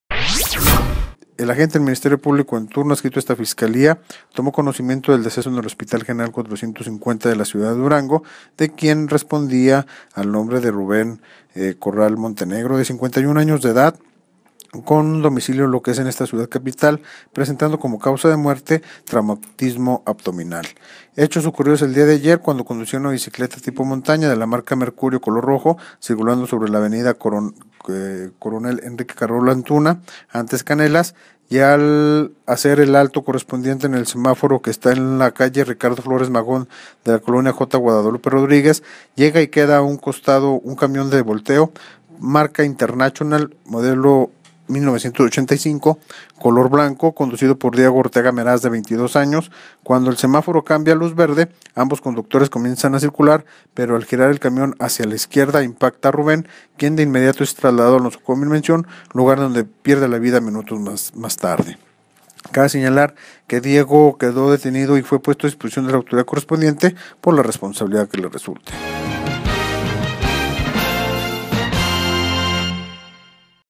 Camión de volteo golpea a ciclista que muere en el hospital
Tras conducir una bicicleta tipo montaña, de la marca Mercurio, color rojo, por la avenida Coronel Enrique Carrola Antuna (antes Canelas), y al hacer el alto correspondiente en el semáforo que está en la calle Ricardo Flores Magón de la colonia J. Guadalupe Rodríguez, a un costado un camión de volteo, de la marca International, modelo 1985, color blanco, avanzó al encender la luz verde y giró hacia la izquierda, impactando al ciclista y tirándolo al piso, pasando sobre él.

Finalmente Rubén Corral Montenegro, de 51 años de edad, por un traumatismo abdominal, dejó de existir cuando era atendido por los galenos en el Hospital General 450 de la ciudad de Durango. El chofer del camión llamado Diego Ortega Meraz, de 22 años, quedó detenido y fue puesto a disposición de la autoridad correspondiente, hasta que se deslinden responsabilidades.